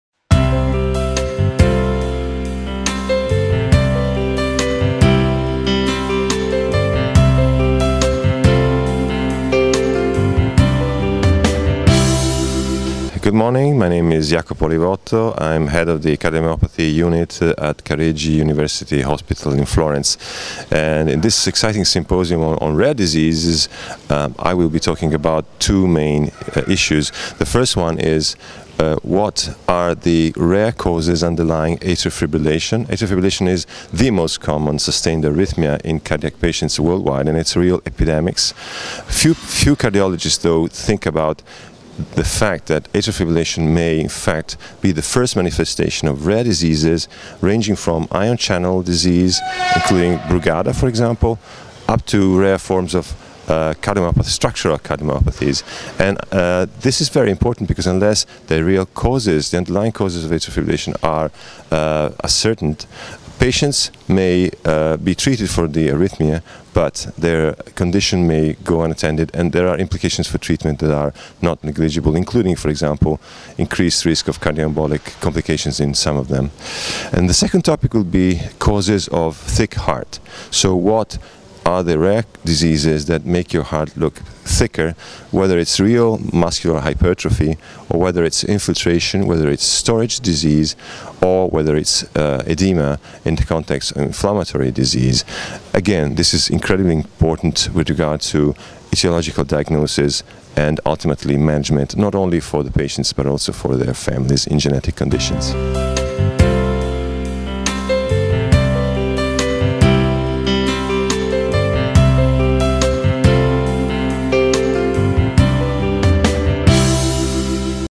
Iacopo OLIVOTTO: "What are the causes of a thick heart?"
International Training Course on
Rare  &  Inherited   Cardiovascular Disease
April 27TH - 29TH, 2017 • ATHENS,  GREECE

Iacopo OLIVOTTO
(Florence, Italy)
Is atrial fibrillation a mask for rare disorders?
What are the causes of a thick heart?